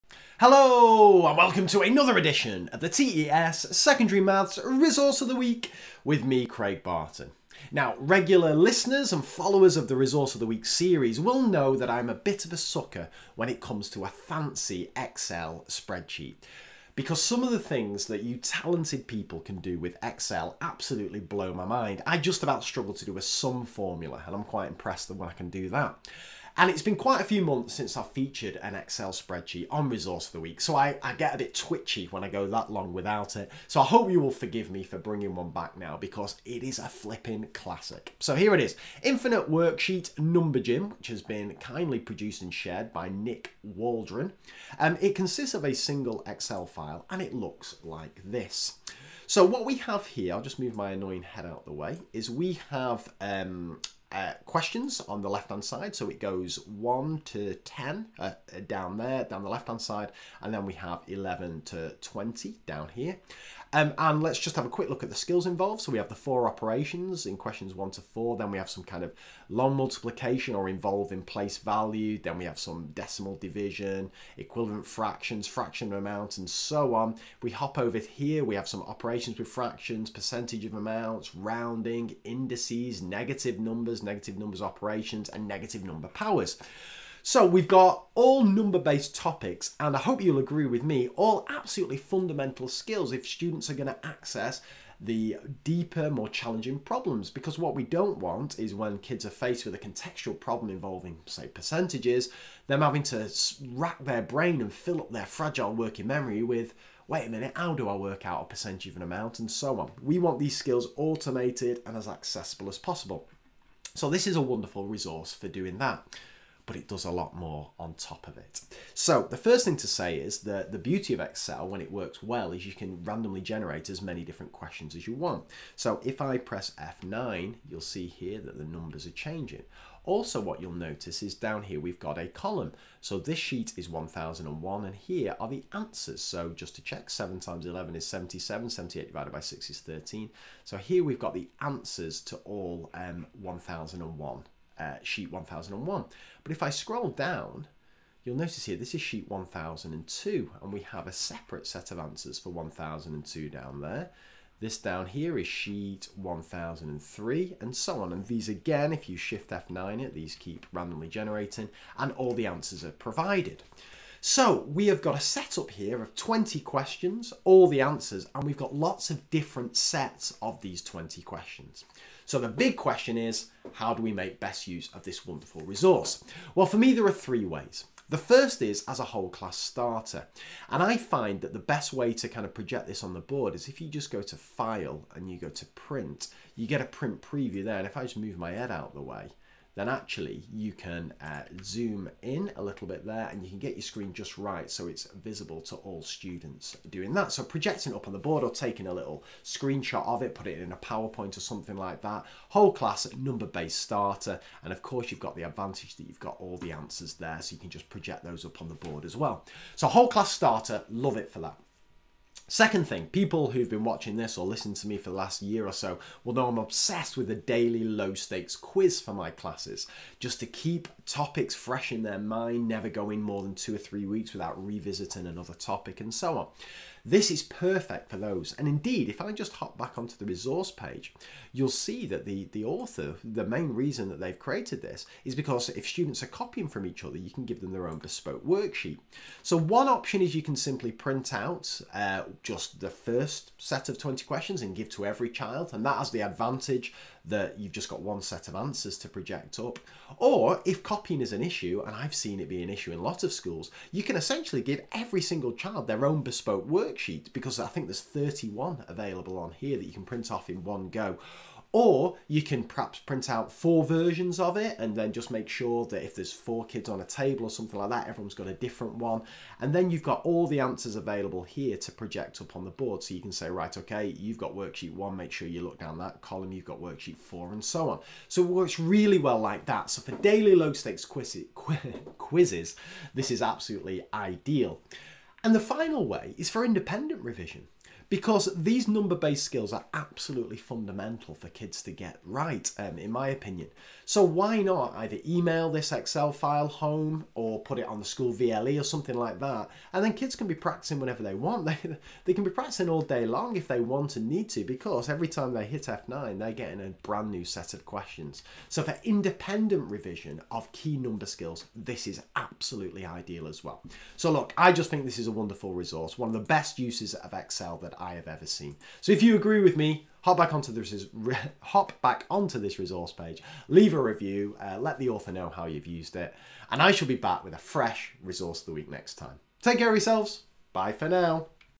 Infinite worksheet - Number Gym - TES Maths Resource of the Week
Regular readers of the Resource of the Week feature will know I am a bit of a sucker for a fancy use of Excel, and once again I have been hooked in. The fact that Excel can be programmed - assuming you know how! - to make use of random numbers in calculations means that essentially you can generate an infinite number of questions. And this resource exploits that beautifully. Here we have 20 number-based questions covering a range of topics, including the four operations, fractions, rounding and powers, and hitting F9 produces a completely different set. And if that wasn’t enough, the answers are also there too!

How can it be used?
There are three ways I will be using this resource:
1. A whole class starter - simply project the set of 20 questions up, and set the students away. A diet of these questions once or twice a week keeps all those key numbers skills ticking over
2. Individual low-stakes quizzes - the author explains that he created this resource to prevent students copying off each other. Indeed, if you scroll down the Excel sheet you will see that there are in fact 32 sets of the 20 questions, all completely different. So, if you click “print” you get 32 personalised quizzes which can be given out to students. Alternatively, I have found that printing out 4 different quizzes, and ensuring every group of four students has a different quiz, works really well. Once students have completed their own quiz, they can then be encouraged to check each other’s work, comparing similar questions.
3. Independent study - emailing this Excel document home, or putting it on the school VLE means that students can benefit from this all important practice whenever they like.

Thanks so much for sharing
Craig Barton

To access the resource and join in the discussion, or to view all the other Maths Resources of the Week in this series, just